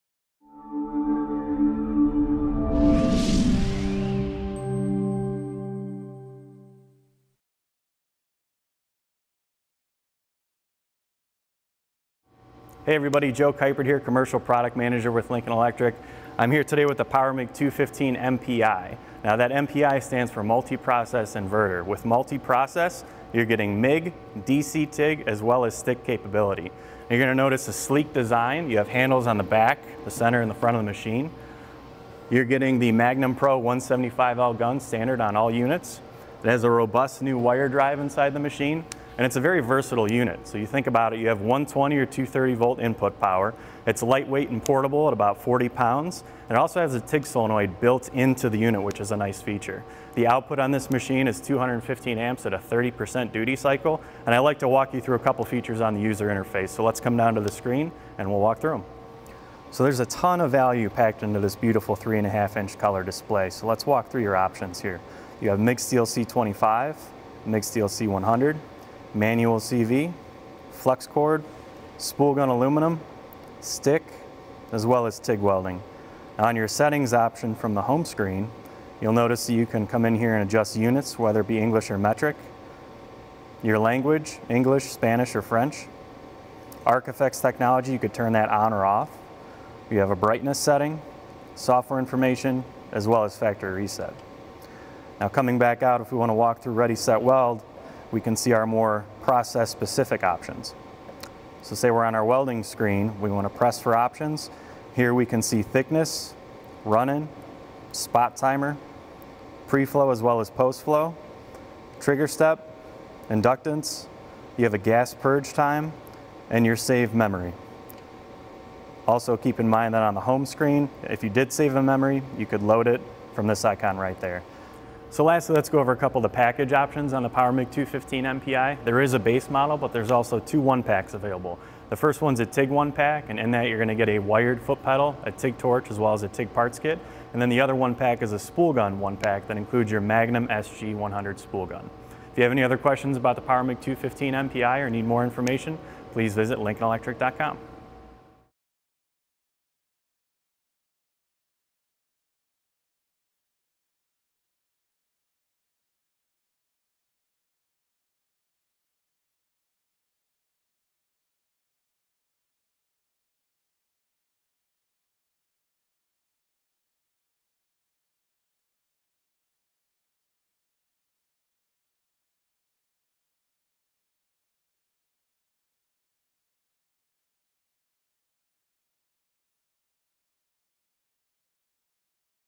Power MIG 215 MPi Multi-Process Welder Set Up
The Lincoln® POWER MIG® 215 MPi® is a 220 amp multi-process welder that is capable of MIG, Stick and DC TIG welding. It operates on standard household 120 volt or 230 volt input power and comes complete with everything you need to MIG and Stick weld, except the MIG shielding gas and stick welding electrodes. TIG torch and accessories are sold SEPARATELY.
Ready.Set.Weld™ feature removes the guesswork in setting the right welding parameters for your job. Large color display guides you through your specific welding application to ensure a great weld every time. ArcFX® shows how your machine settings will affect the weld outcome, and you can save your preferred settings to get welding faster.
The POWER MIG 215 MPi performs MIG, Flux-Cored, Stick and DC TIG welding and is perfect for the farm, small welding contractor or DIY welder. It has built in spoolgun circuitry, for quality aluminum MIG welds using the OPTIONAL Magnum® PRO 100 SG spoolgun. The 215 MPi produces up to 220 amps of MIG welding output, 175 amps of DC TIG welding output and 175 amps of stick welding output. It plugs into a 120V or 230V outlet and has a three year factory warranty.